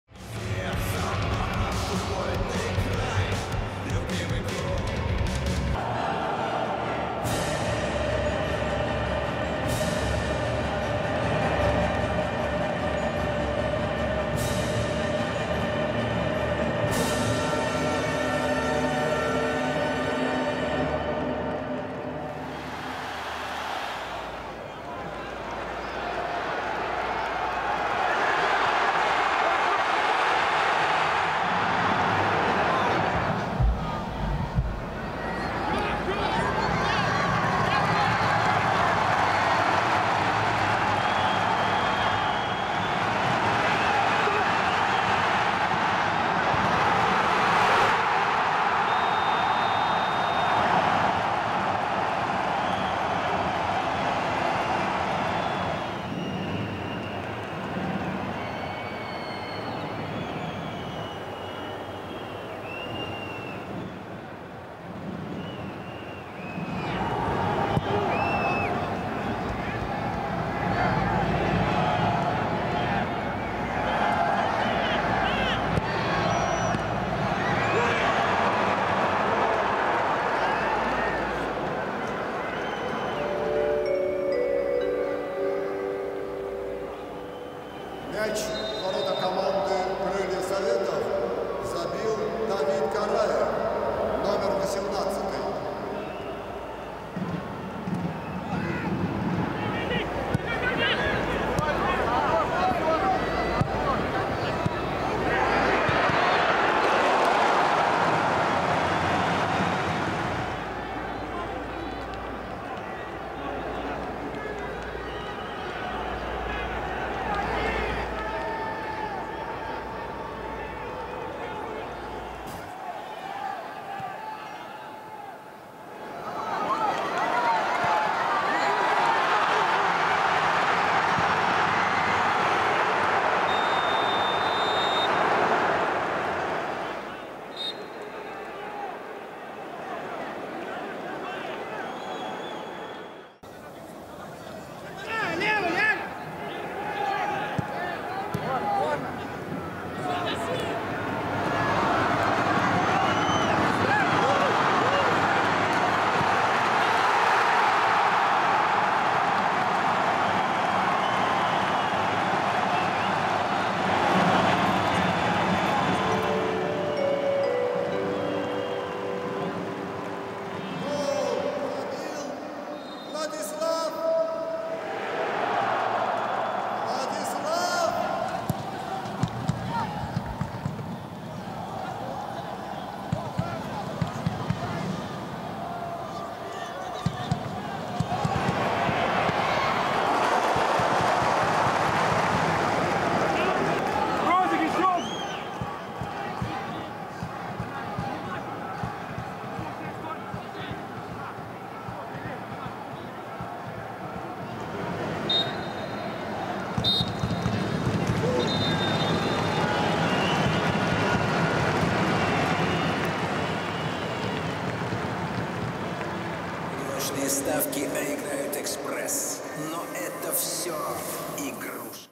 Highlights Krylia Sovetov vs Torpedo (1-1) | RPL 2022/23
🤝 A draw in Samara (1-1)

Goals: Shitov 68’ – Karaev 20’

Krylia Sovetov: Lomaev, Gaponov, Gorshkov, Soldatenkov, Bijl, Vityugov, Sokolov, Ezhov, Kovalenko, Glushenkov, Tsypchenko

Torpedo: Botnar, Samsonov, Smolnikov, Kozhemyakin, Ryazantsev, Pomerko, Sultonov, Netfullin, Enin, Karaev, Kalmykov